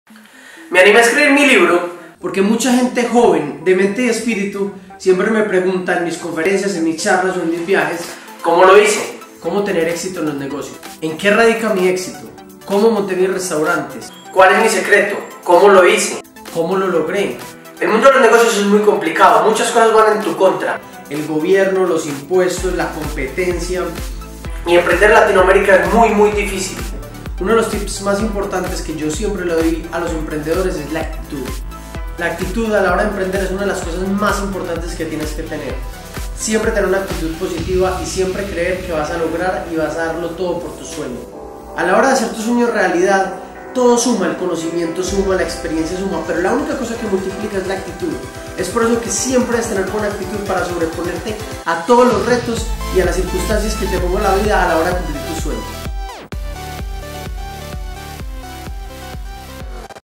La Receta de Juanma Actitud
Mi receta más importante está por salir... mi primer tip para ustedes: LA ACTITUD. Contame en que situaciones de tu vida la actitud te ha llevado a ganar algo?? Comenta tu historia abajo ⬇